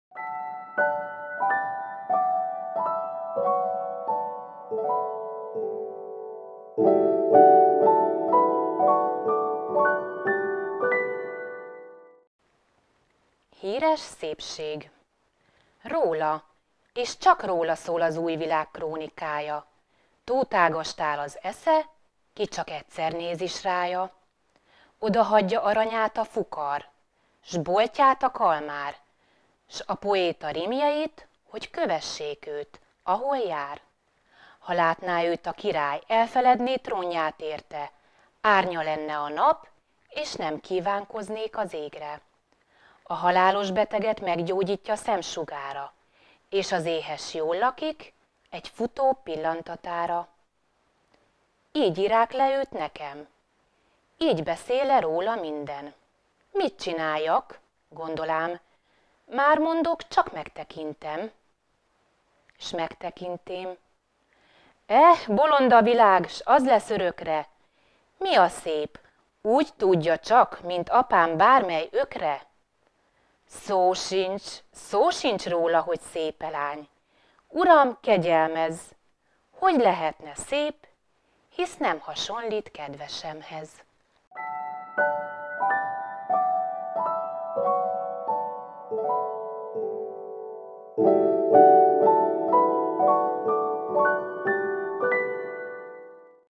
Petőfi Sándor: Híres Szépség - PETŐFI 200 - AZ ÉLŐ KÖLTŐ
Könyvtárunk csatlakozott a Levéltári PETŐFI 200 - AZ ÉlŐ KÖLTŐ programjához.
A verset előadja: Tóthné Ráski Dóra könyvtáros